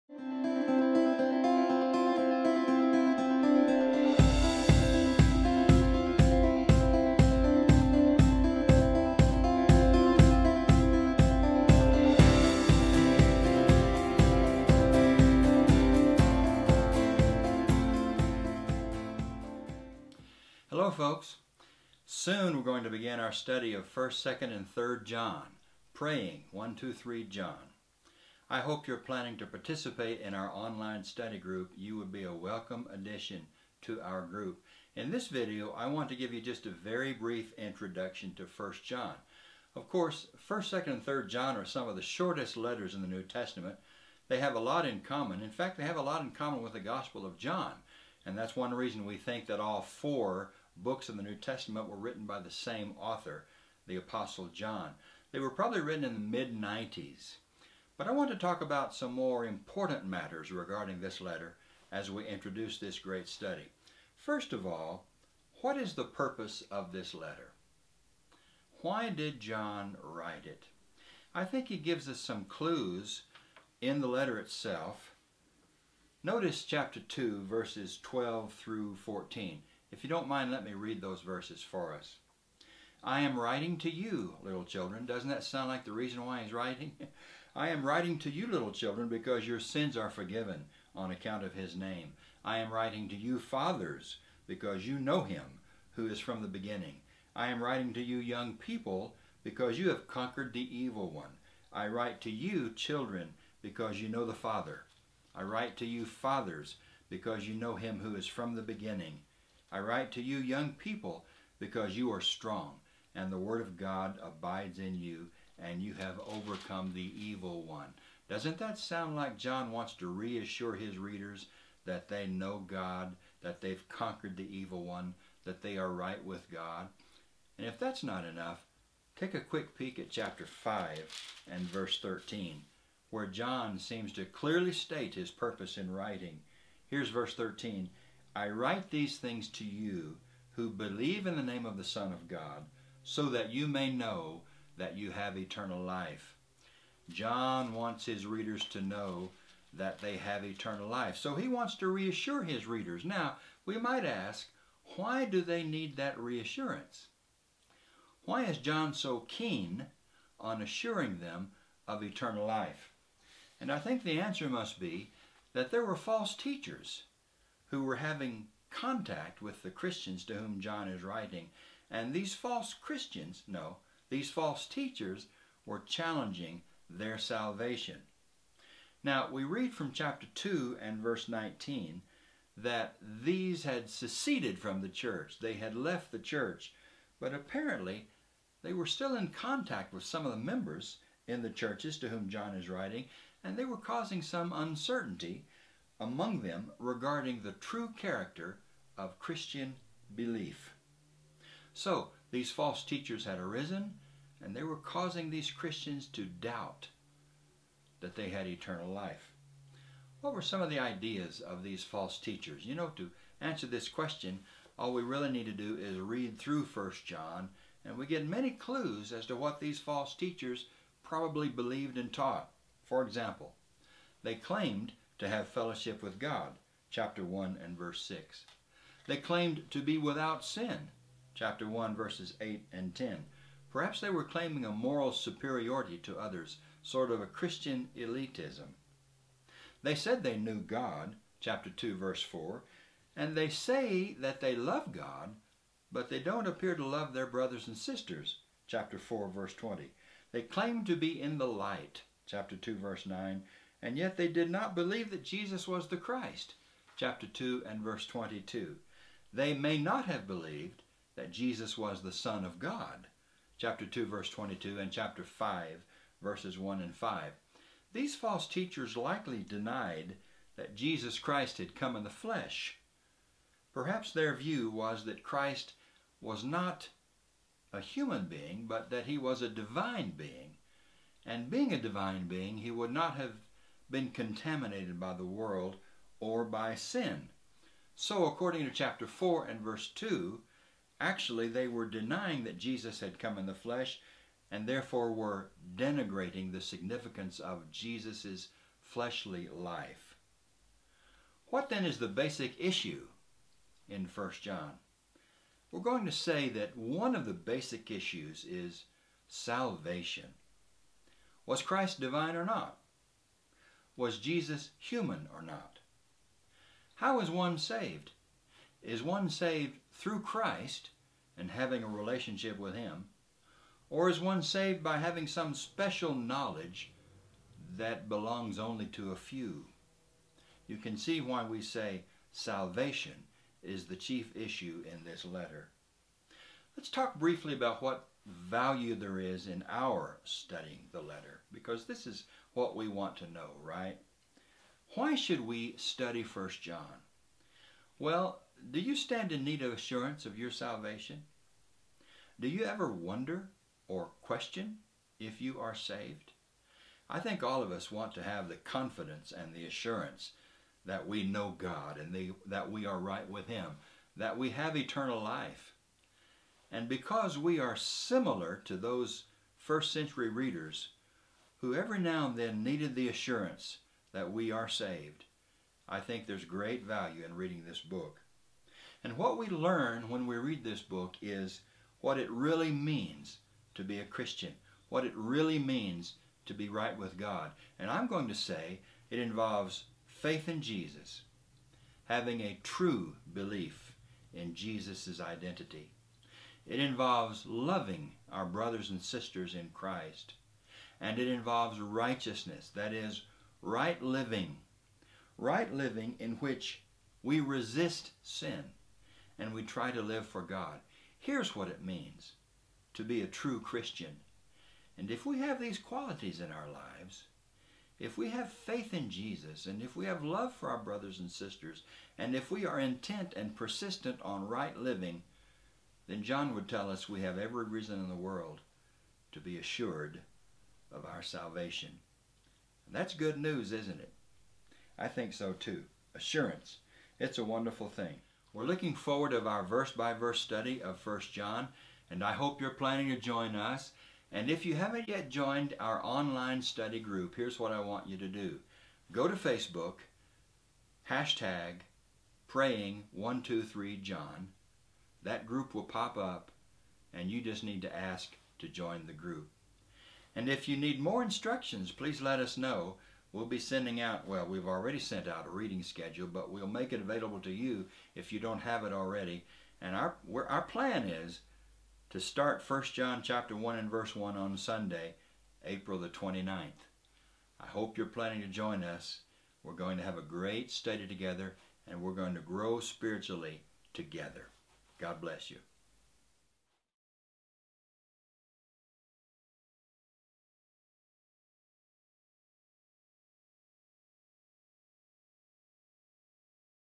praying123john Introduction
Join us as we begin a new study through 1st John!